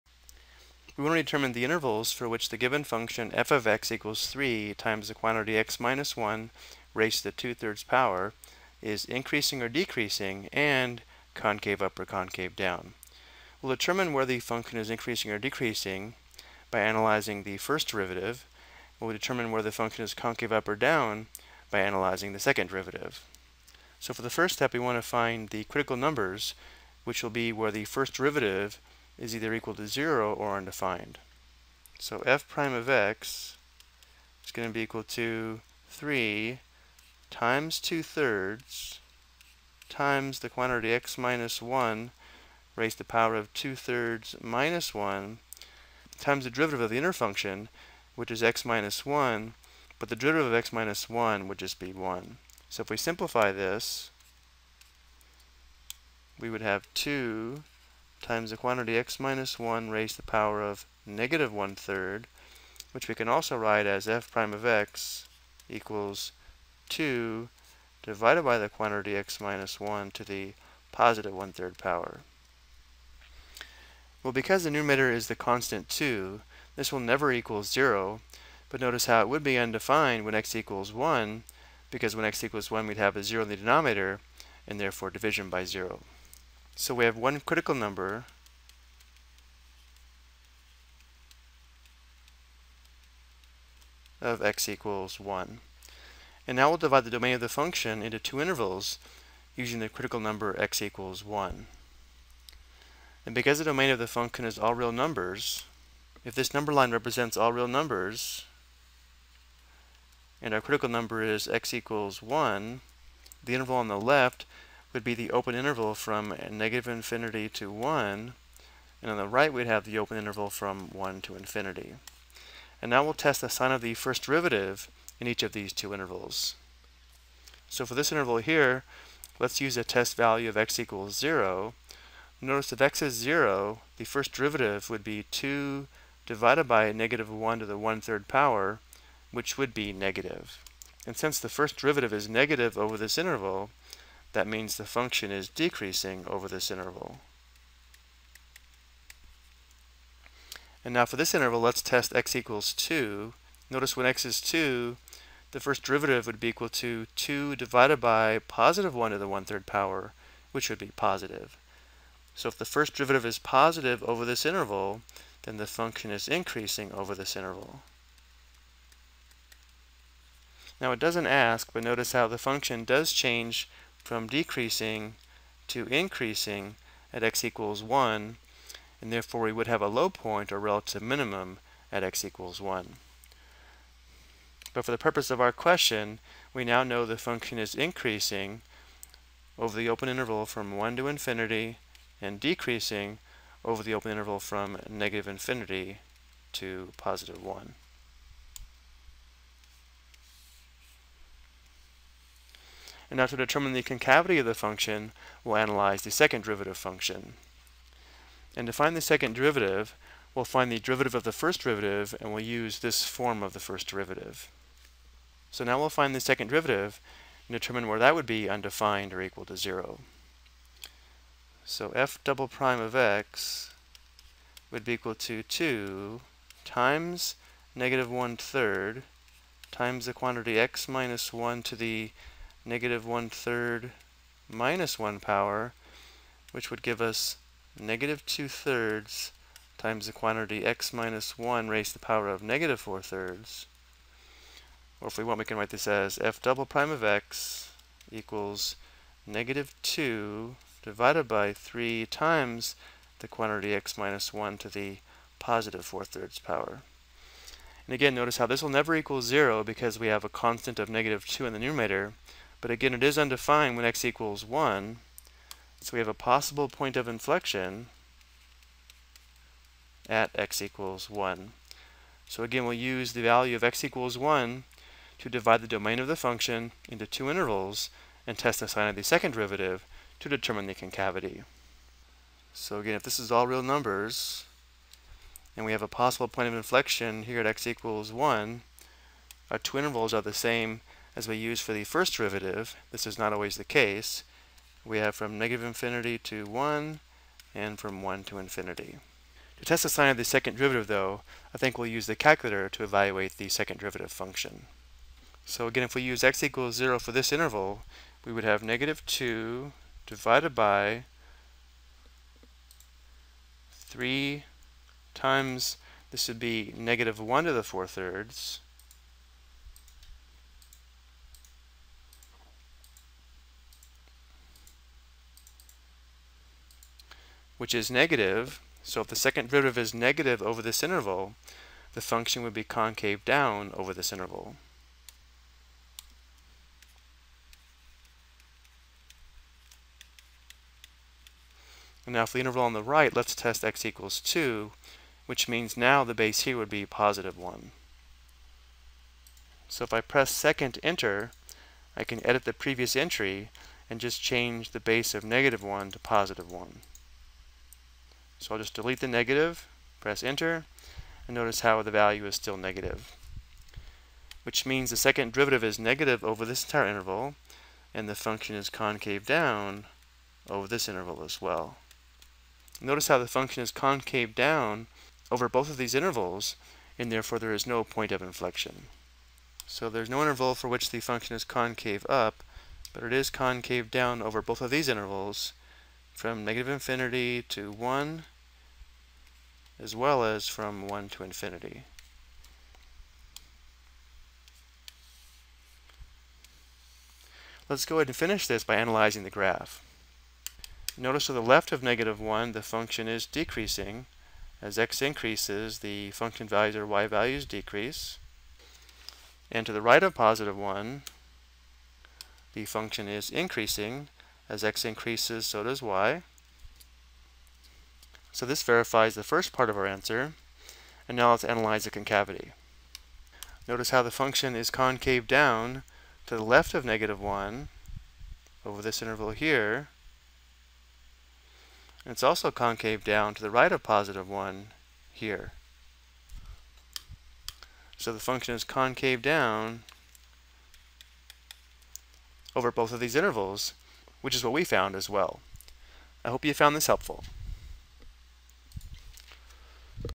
Ex: Find the Intervals a Function is Increasing/Decreasing/Concave Up or Down - Rational Exponent
This video provides an example of how to find the intervals a function with a rational exponent is increasing or decreasing and concave up or concave down.